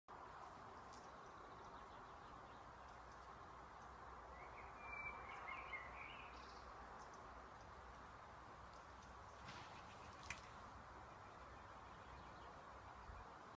Fågelskrämma??
Sebbe en svart och vit Engelsk Springer Spaniel jagar fåglar🕊